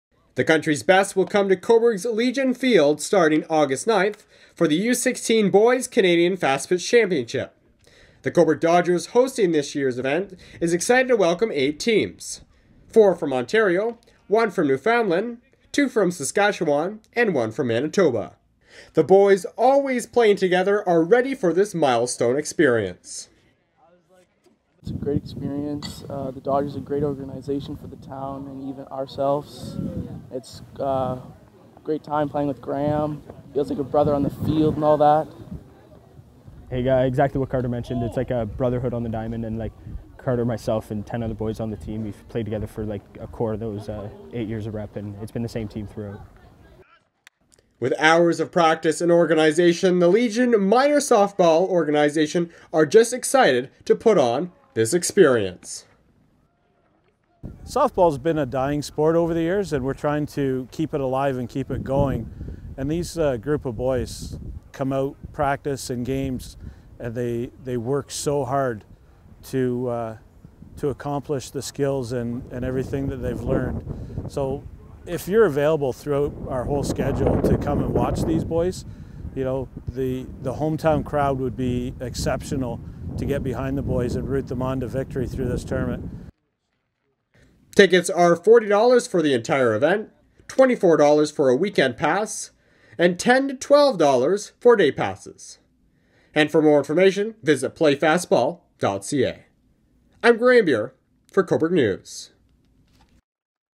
Cobourg Dodgers to Host National Fast Pitch Tournament
The Cobourg Legion Minor Softball Organization is hosting 8 teams from all over the country. From August 9th to the 13th this town will see great softball action. Graham Beer reports.